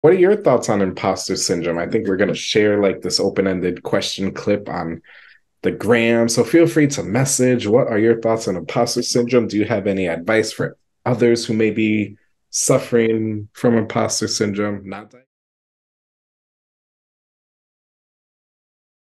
What Are Your Thoughts About Imposter Syndrome for Pole Dancers?
Have you experienced imposter syndrome as a pole dancer?

Or do you have advice for pole dancers who may be experiencing imposter syndrome?